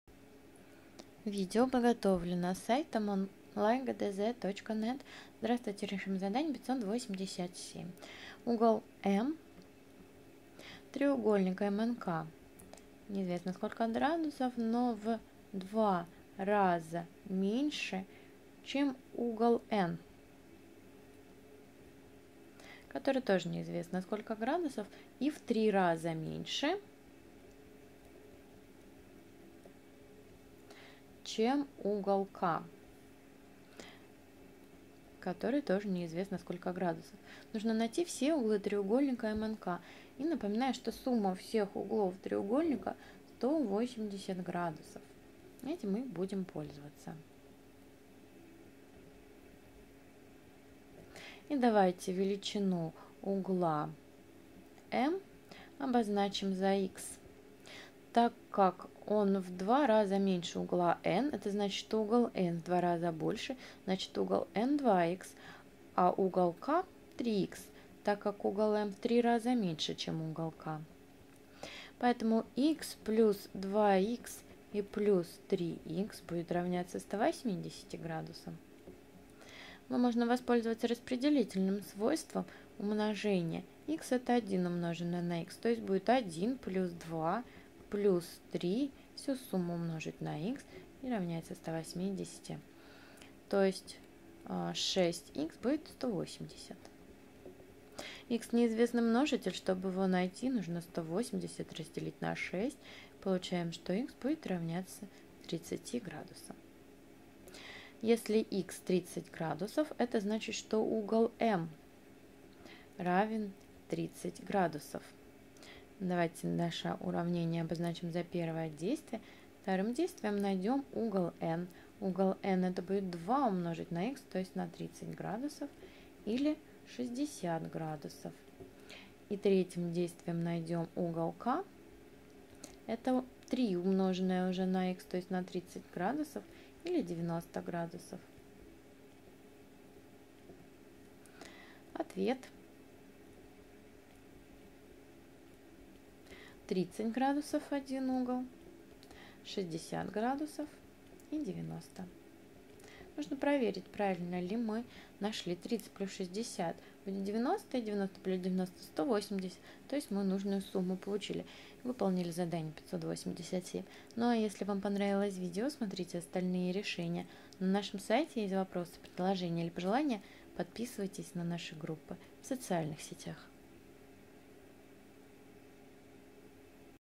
Задание № 587 - Математика 5 класс (Зубарева, Мордкович)
В задании 587 вам нужно найти все углы треугольника, если не даны величины ни одного из углов, а даны только соотношения между углами. Чтобы сделать это задание без ошибок, воспользуйтесь нашим видео с решением.